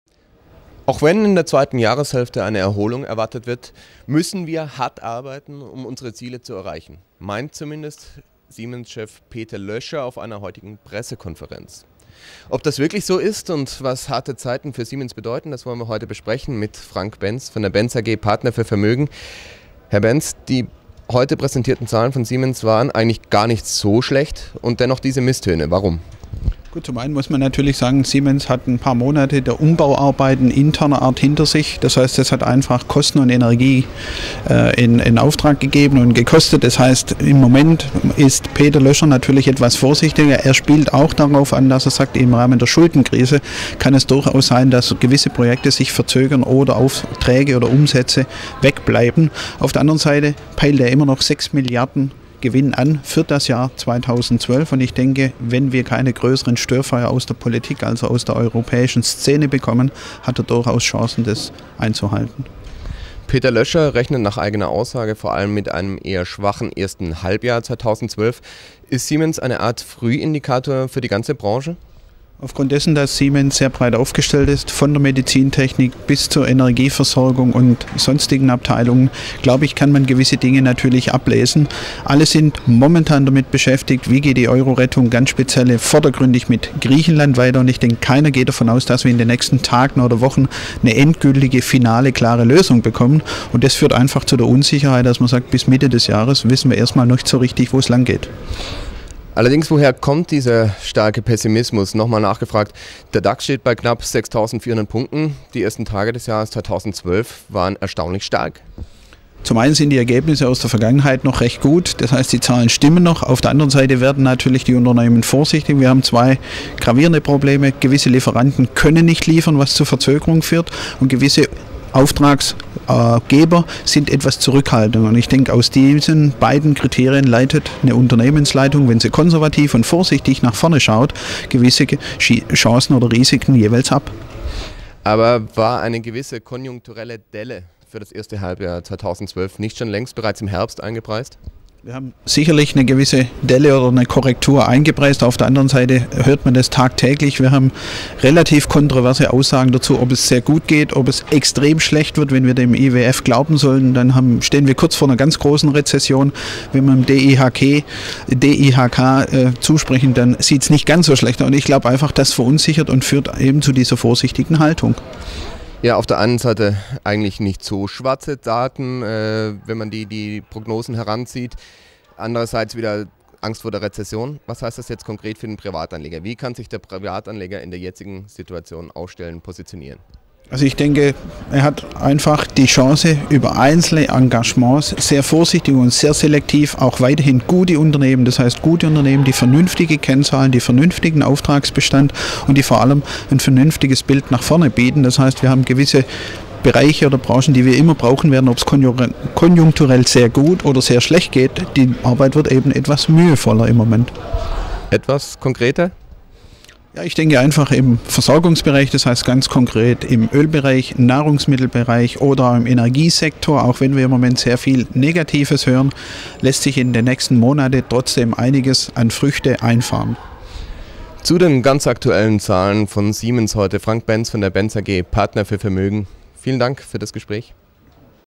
Siemens: Hiobsbotschaften auf Hauptversammlung
„Auch wenn in er zweiten Jahreshälfte eine Erholung erwartet wird, müssen wir hart arbeiten, um unsere Ziele zu erreichen", so Siemens-Chef Peter Löscher heute auf der Hauptversammlung des Industriekonzerns. Siemens stimmt seine Aktionäre also bereits auf harte Zeiten ein. Was bedeuten die schwachen Siemens-Zahlen für die Mitkonkurrenten am deutschen Markt? Frank Benz, Benz AG "Partner für Vermögen" mit einer ersten Einschätzung im TV-Interview